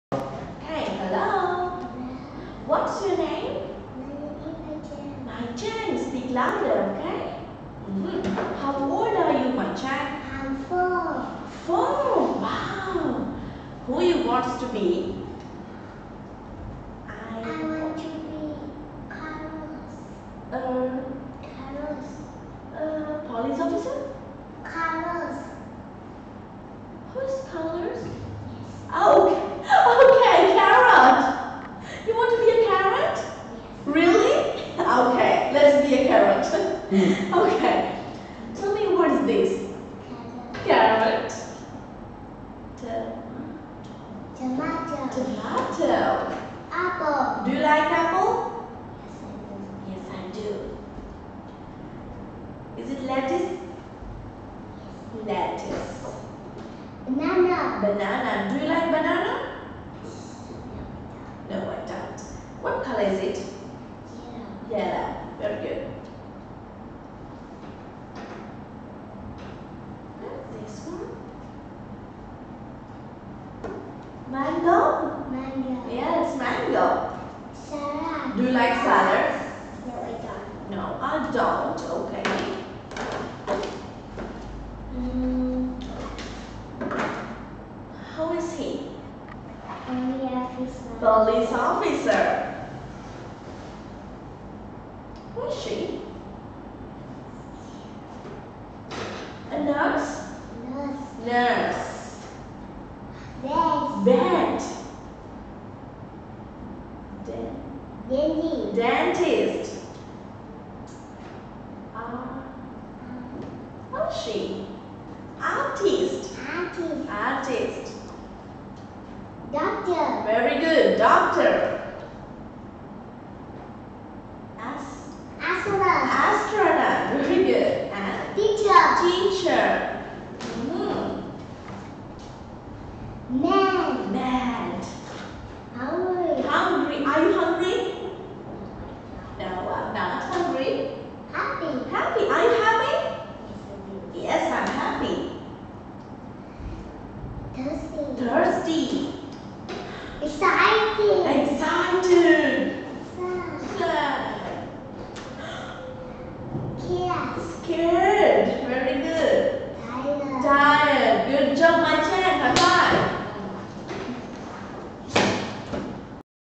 Mai Trang PreS3 ( AMES Hà Đông )
Bài nói của bạn Mai Trang lớp Pre - S3 chi nhánh Hà Đông